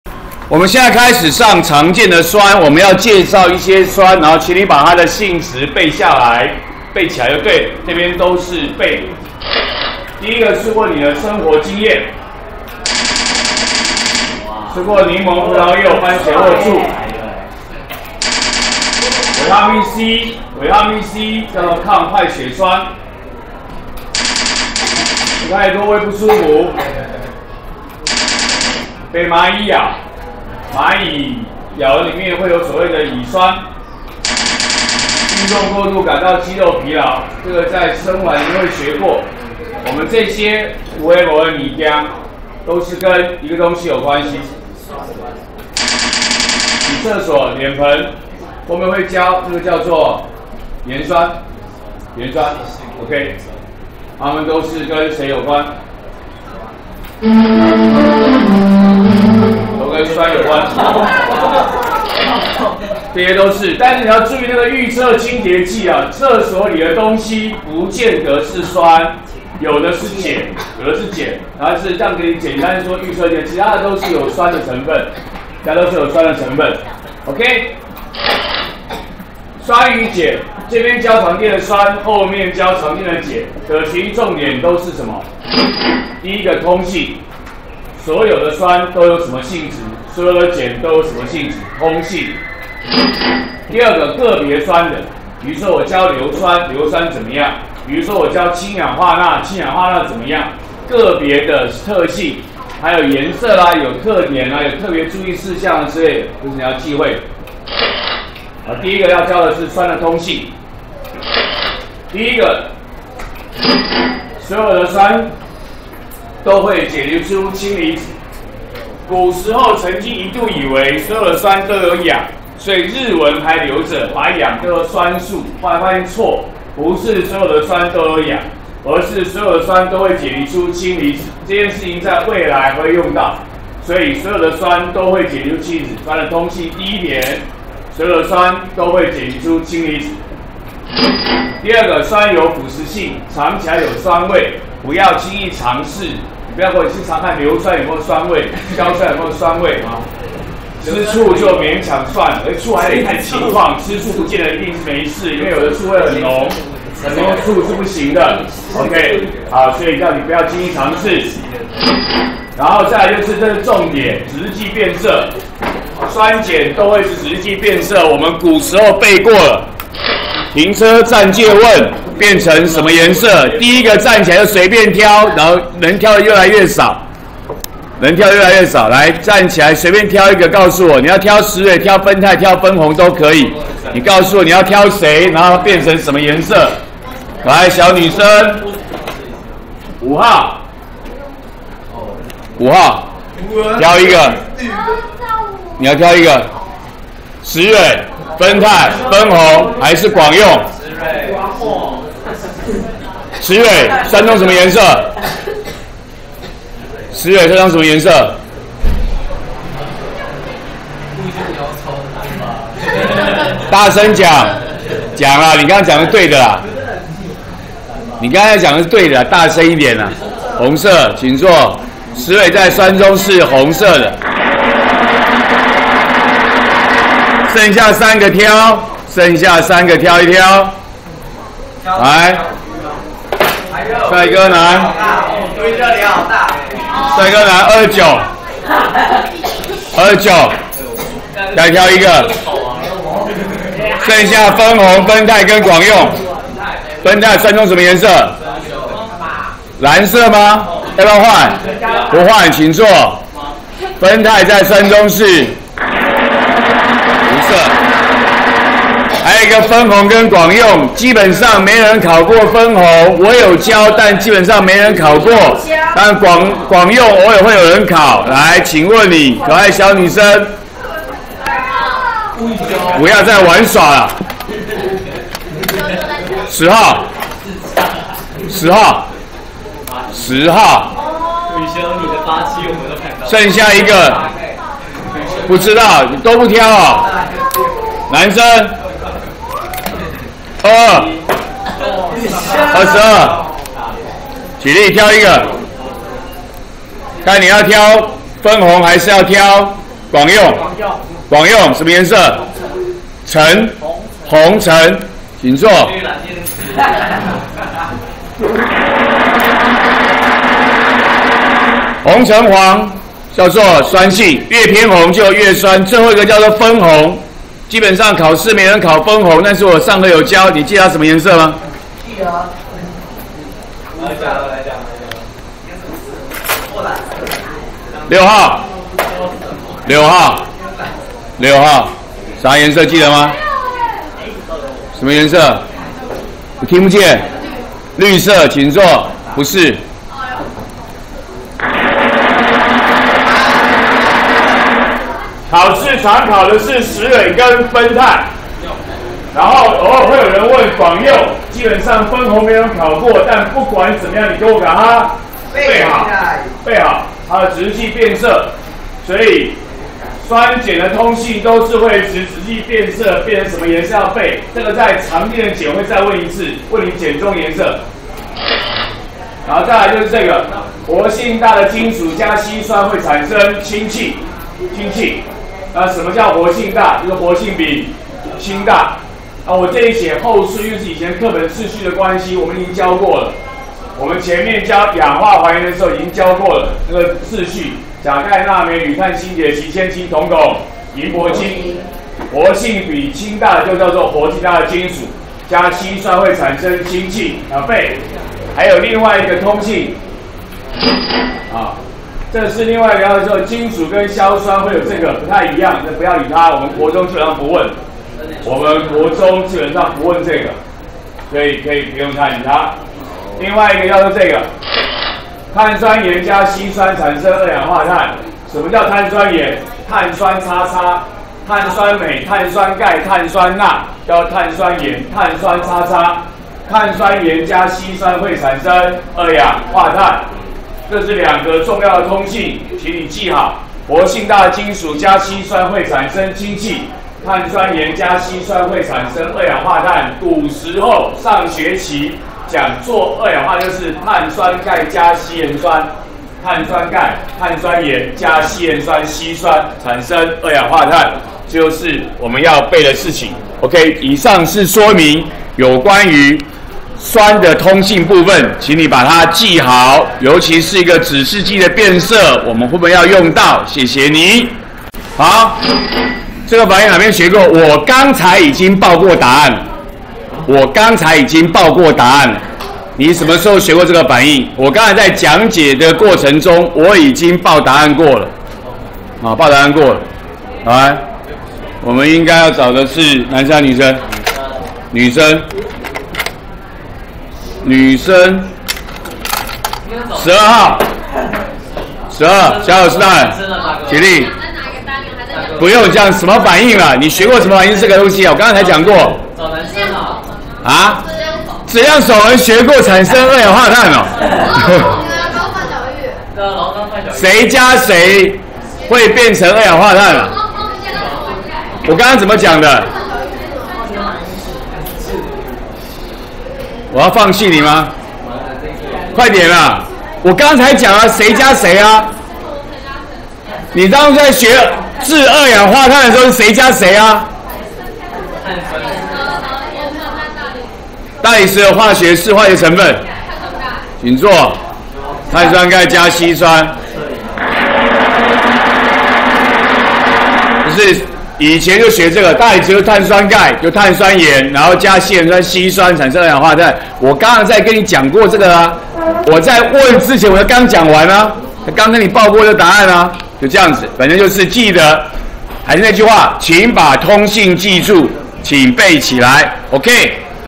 104學年國二理化＿酸鹼鹽09：酸的通性
介紹生活經驗中的酸。介紹酸的通性，過程中詢問同學，酸鹼指示劑在酸中顏色。